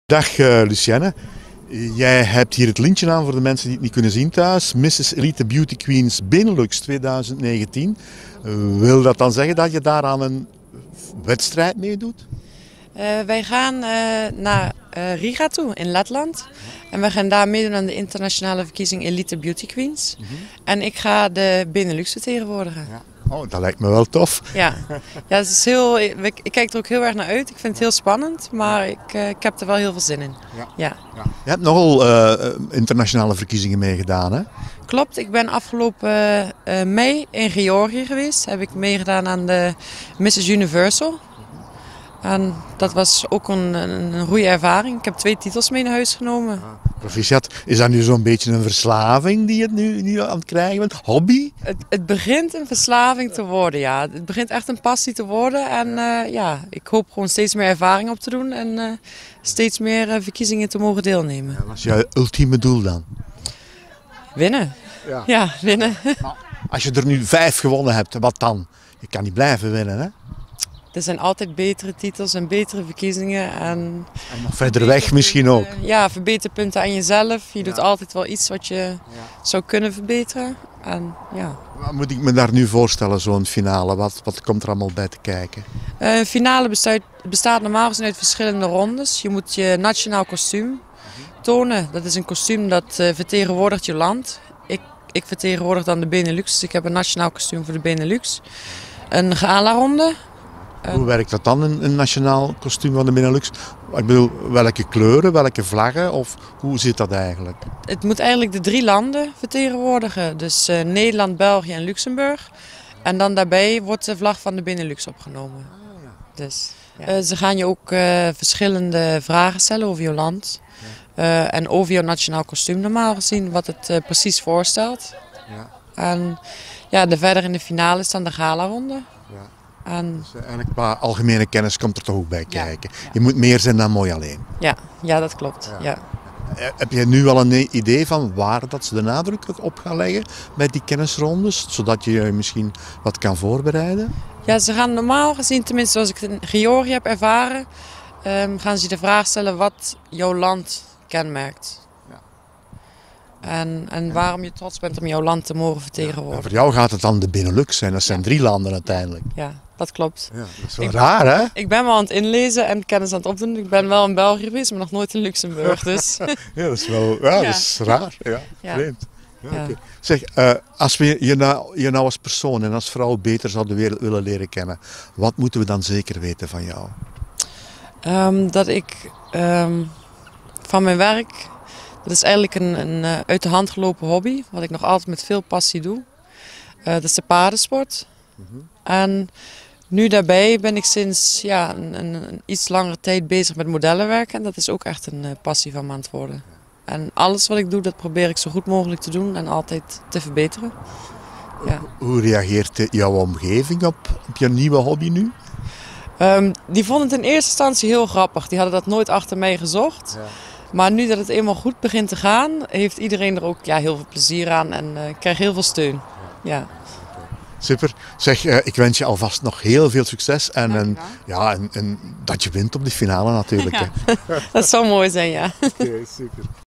Lucienne de Winter Mrs Elite Beauty Queen Benelux 2019
Lucienne vertegenwoordigt eind deze maand de Benelux in Riga, Letland. Intussen zijn verkiezingen een passie geworden waarbij ze altijd maar meer ervaring wil opdoen.Lucienne zit in de paardensport, dat is haar werk en tevens haar hobby. En sinds enige tijd is ze passioneel begonnen met modellenwerk.
In België is Vicky Sagang director van Elite Beauty Queens waarvan de internationale verkiezing door gaat van 28 juli tot 2 augustus in Riga Letland.